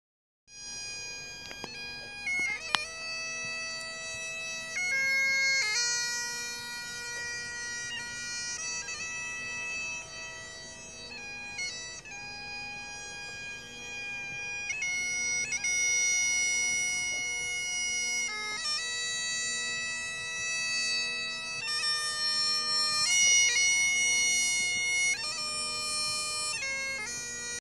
The Piper in the Dark
A piper practicing in the dark at Fairy Meadow Beach. 9 April 2018